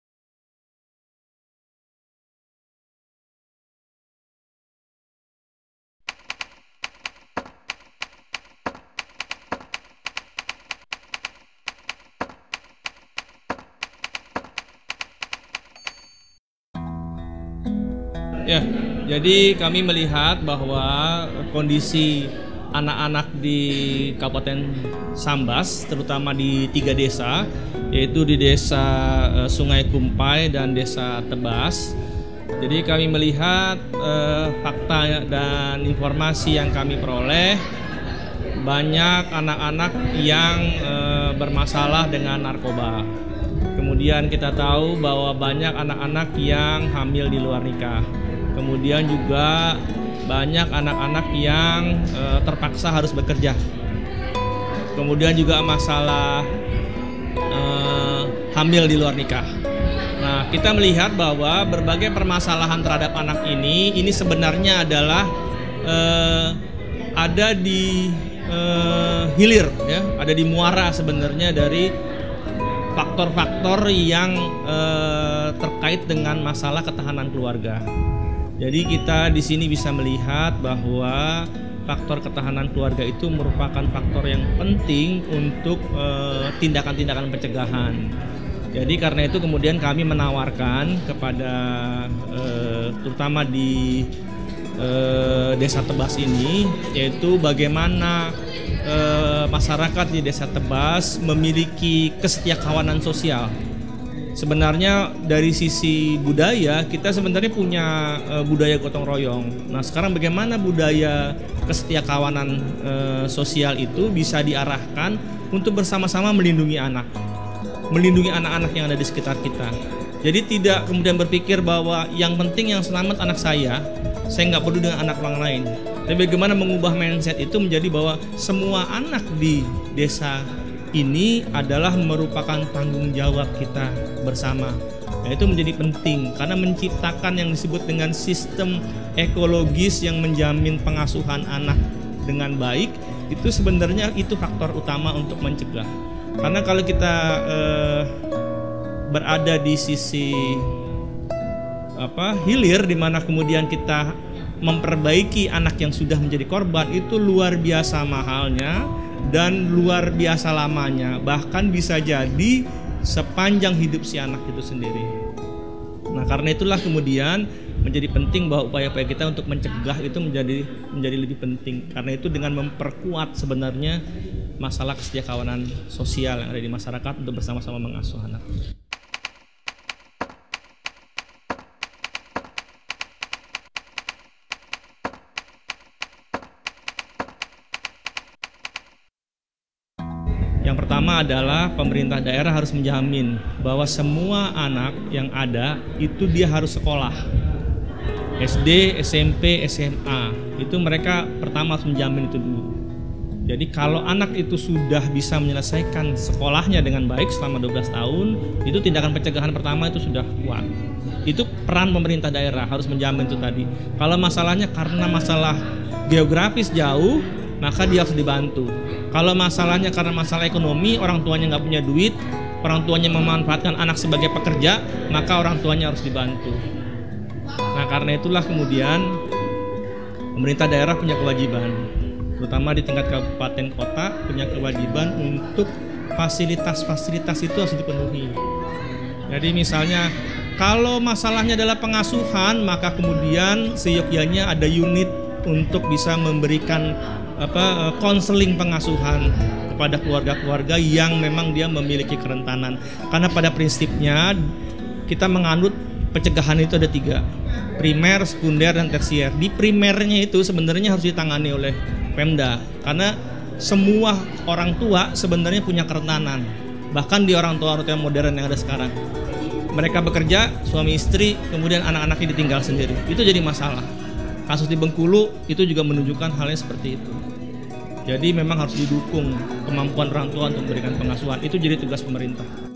STATMENT DEPUTI PERLINDUNGAN ANAK TENTANG FAKTA PERLINDUNGAN ANAK DI KAB. SAMBAS
STATMENT DEPUTI PERLINDUNGAN ANAK TENTANG FAKTA FAKTA  PERLINDUNGAN ANAK YANG TERDAPAT DI KAB. SAMBAS DAN PENJELASAN TENTANG KONSEP "NEGARA HADIR" DI TENGAH PERMASALAHAN PERLINDUNGAN ANAK DI KABUPATEN SAMBAS